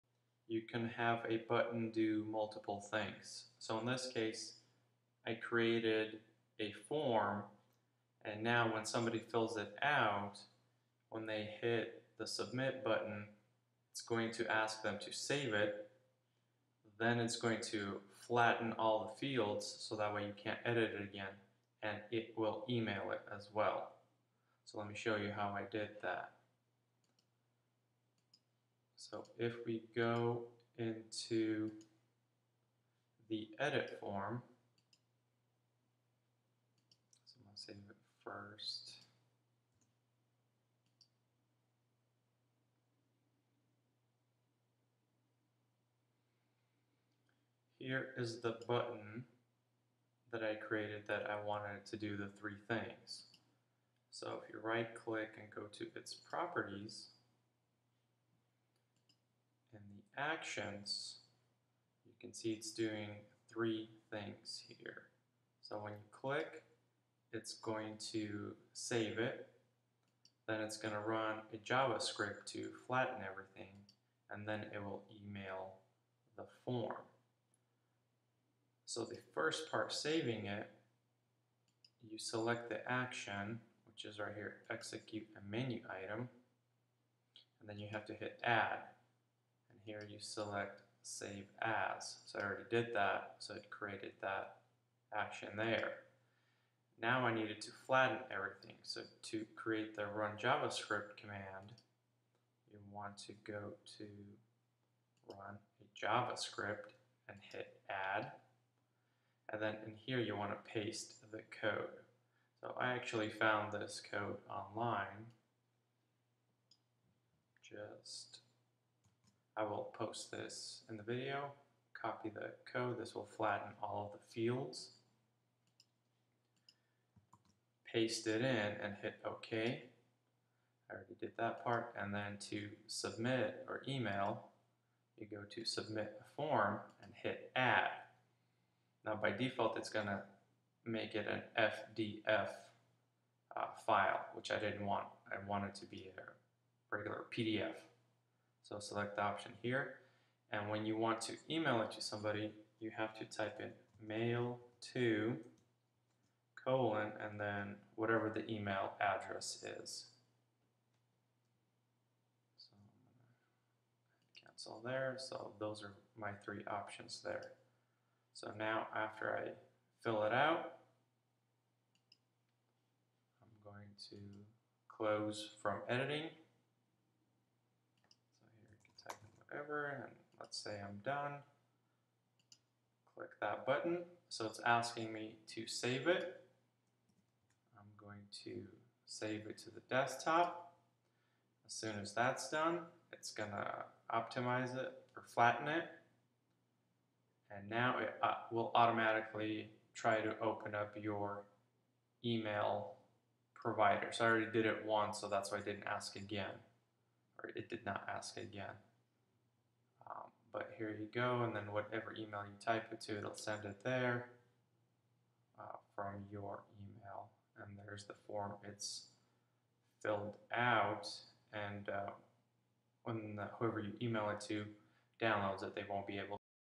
Acrobat Pro - Make PDF button do multiple actions - Save, Flatten, Email
With one click have your PDF do multiple things. In this tutorial it will Save the filled out form, then it will flatten it and email it. I wanted the file flattened so that the person receiving the filled out form would not be able to edit it.
Need to use the Acrobat Pro version to edit the properties.
Here is the javascript code to flatten the text fields.
And yes I know you can do these 3 things easily in different ways but I was testing the actions on one button :)